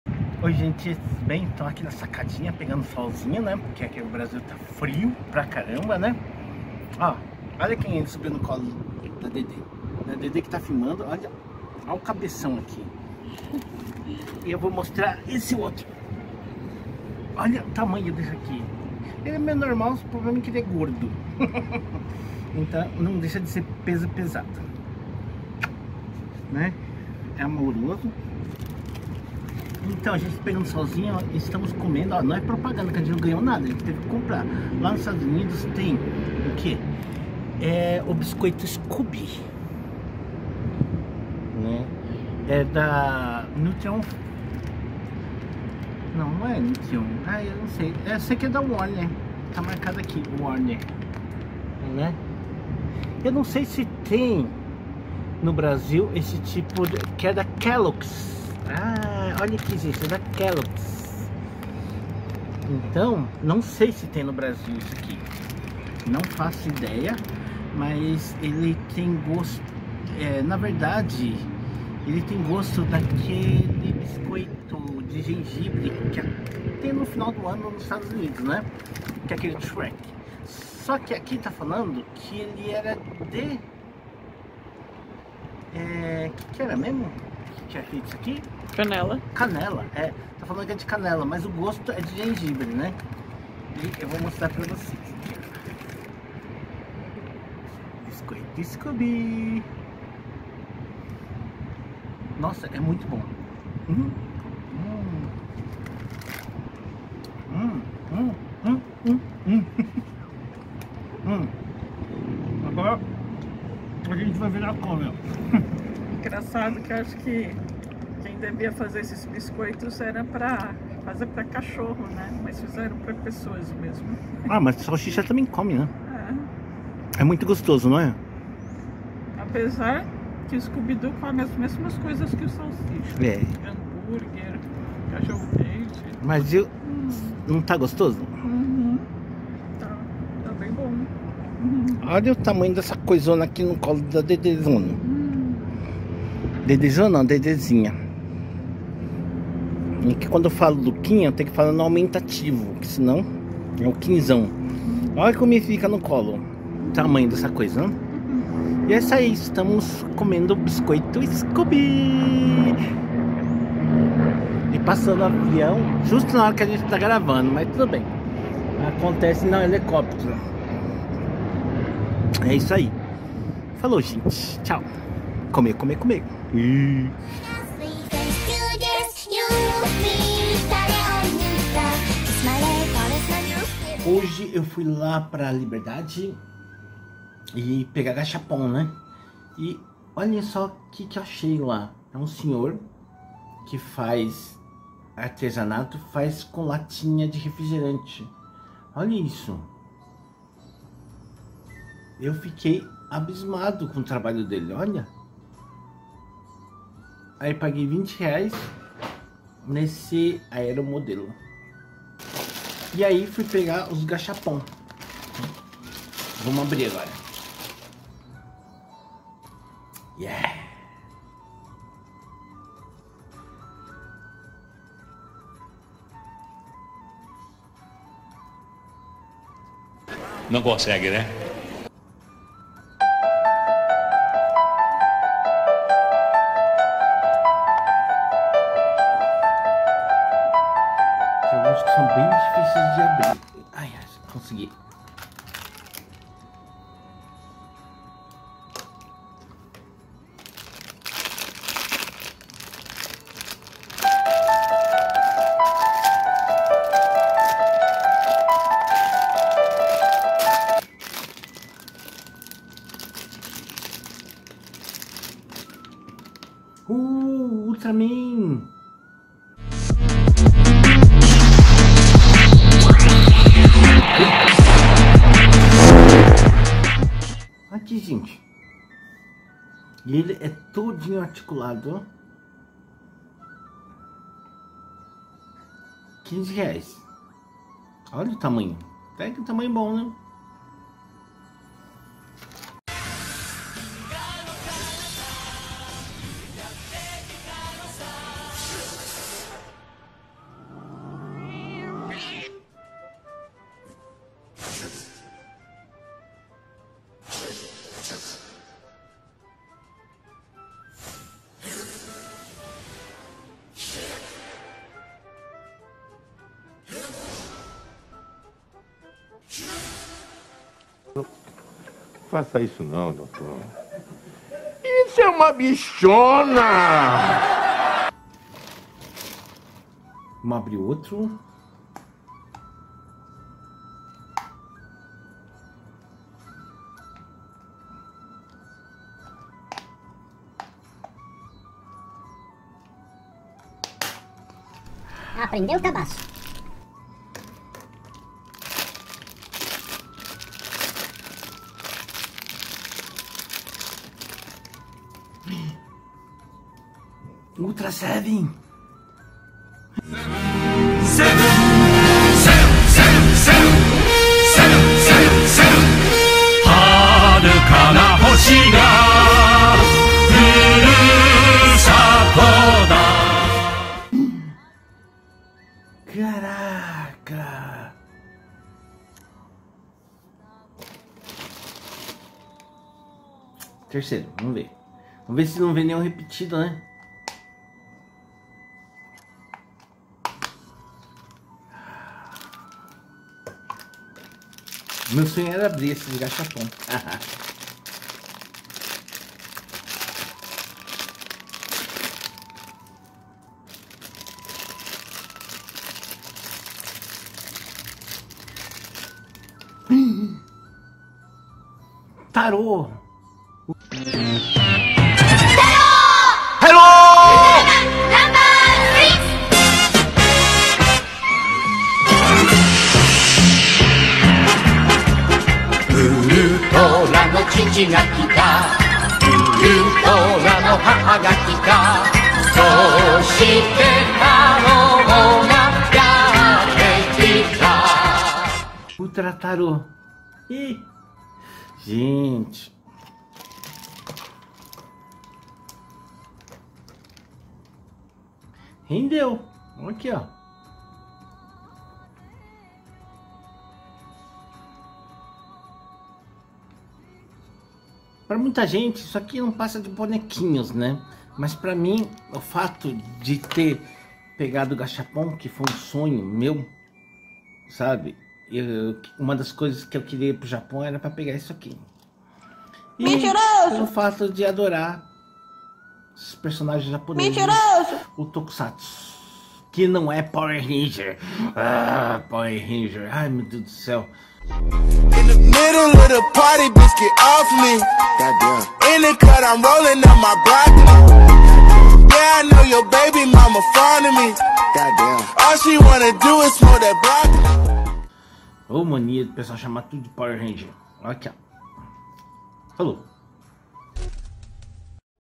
NOVOS GACHAPONS E BISCOITO SCOOBY-DOO
Fui na Liberdade pegar mais Gachapon de ultraman.